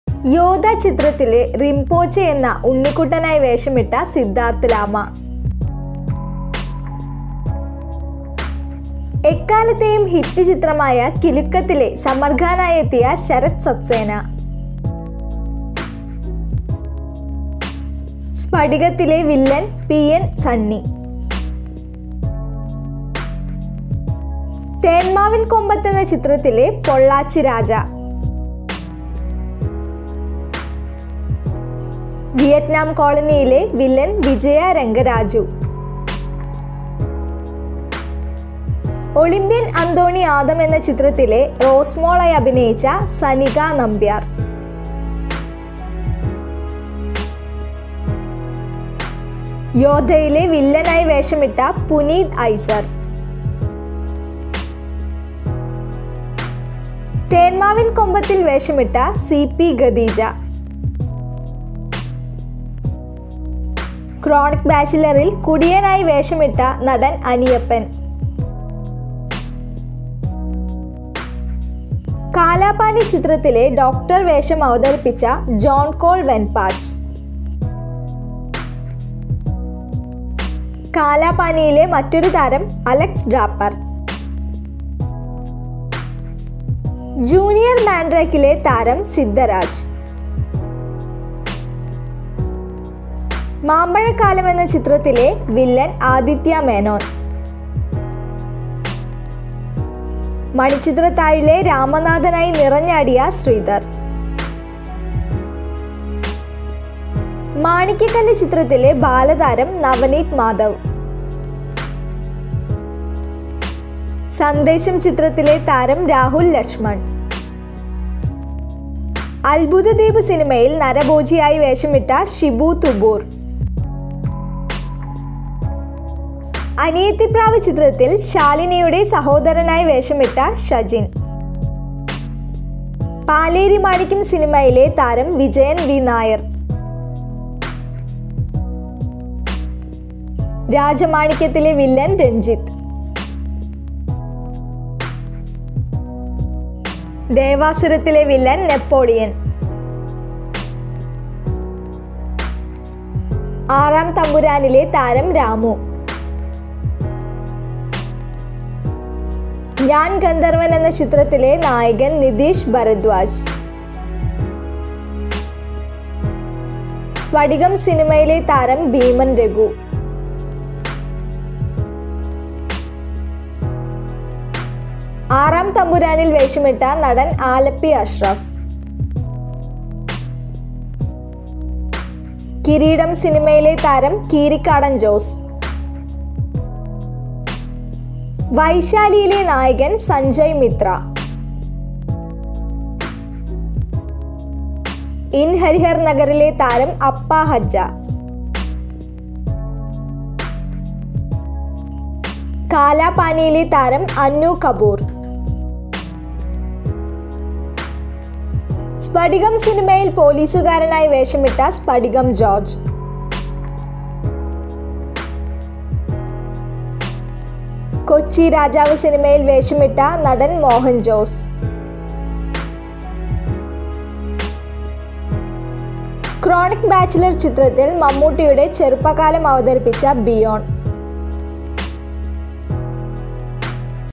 Malayalam Actors: Then and Now | മലയാളത്തിലെ താരങ്ങൾ അന്നും ഇന്നും..
This video shows a list of old Malayalam movie actors and their new look.